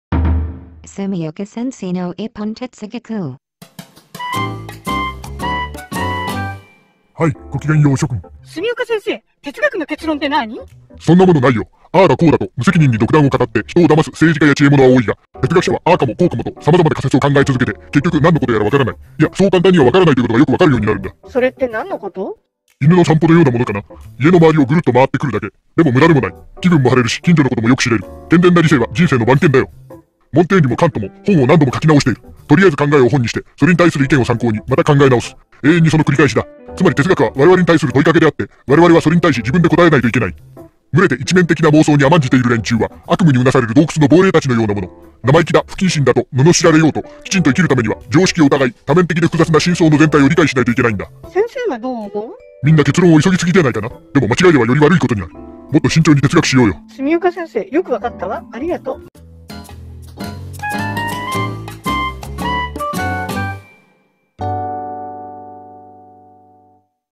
📓哲学の結論: 永遠の問答: 純丘先生の１分哲学！ 60
見るだけで賢くなれるブレインサプリ。大阪芸術大学の純丘曜彰教授博士が哲学のキーワードを１分で解説します。